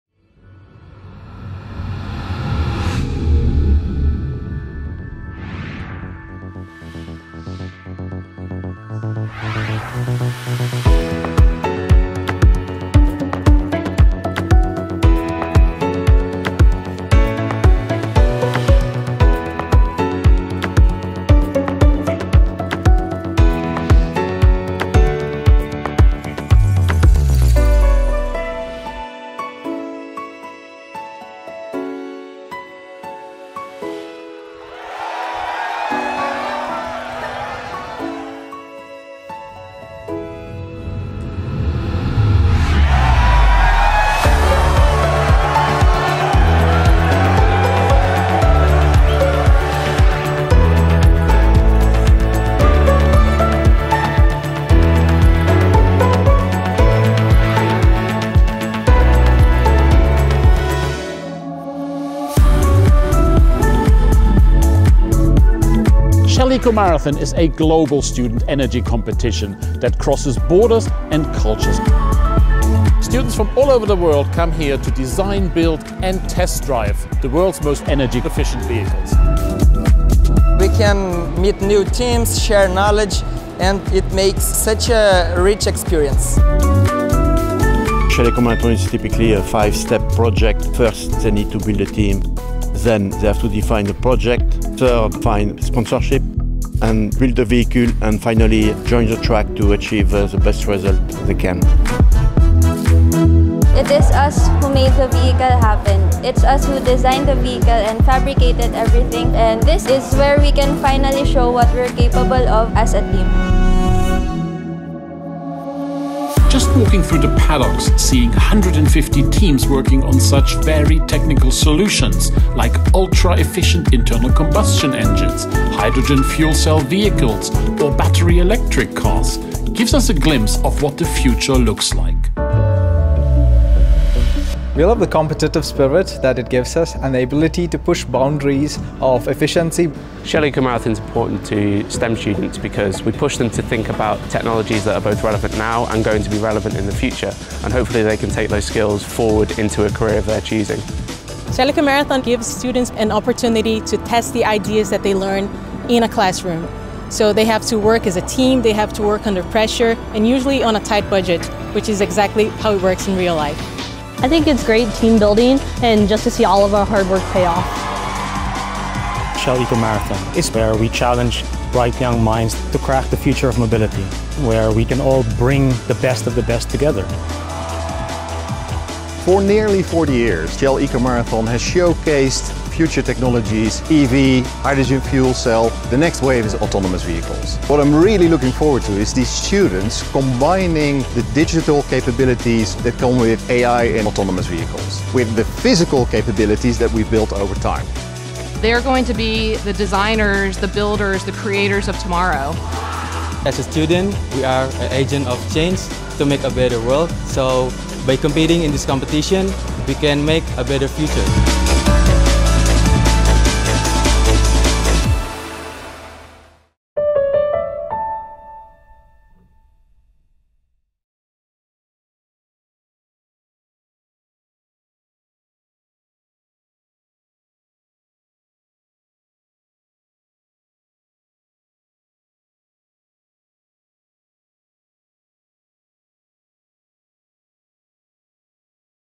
What is Shell Eco-marathon?
Shell Eco-marathon is one of the world’s leading student engineering competitions. Over the past 37 years, the programme has consistently brought to life Shell’s mission of powering progress together by providing more and cleaner energy solutions. The global academic programme brings together Science, Technology, Engineering and Maths (STEM) students from across the globe to design, build and test-drive some of the world’s most energy-efficient vehicles.

👉 Follow Shell Eco-marathon on Instagram: @shell_ecomarathon